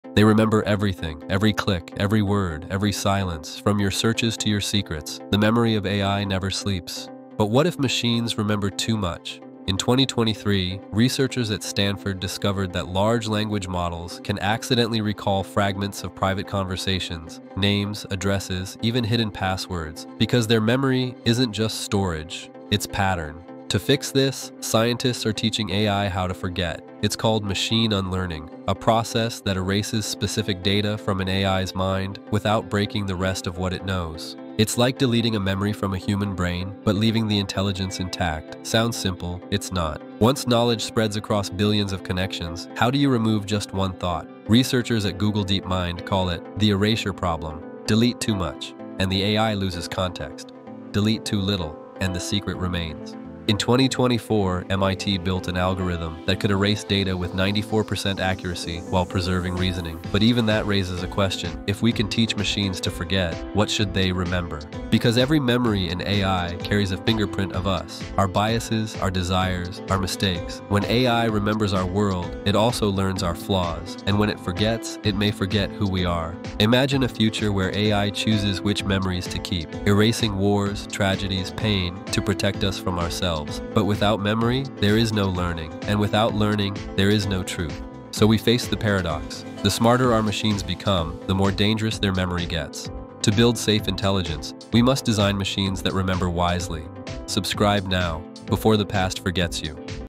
AI's Memory Paradox
Machines never really forget.
Every click, every word, every silence — stored somewhere in the infinite memory of AI.
But when artificial intelligence remembers too much, privacy turns into a paradox.

In this cinematic episode of Power Flow, discover:
⚡ How AI secretly stores your data in neural networks
⚡ What “machine unlearning” really means — and how scientists teach AI to forget
⚡ Why deleting data is harder than erasing human memory
⚡ The truth behind Google DeepMind’s erasure problem

🧠 The smarter our machines become, the more dangerous their memory gets.
Will we control what they remember — or what they choose to forget?